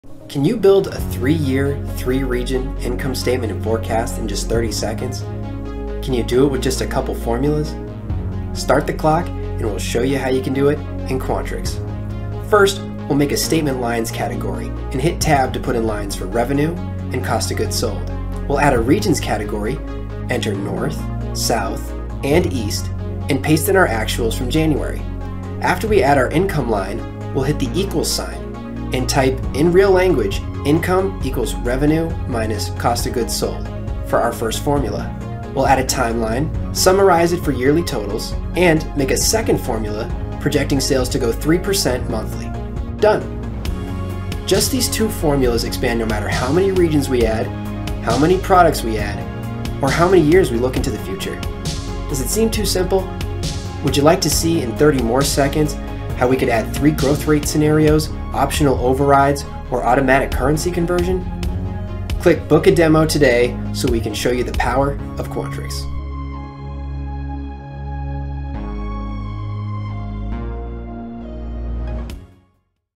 30 Second Quantrix Demo: Income Statement & Forecast
Discover the power of Quantrix Financial Modeling in only 30 seconds!

In this video, we build an income statement and forecast.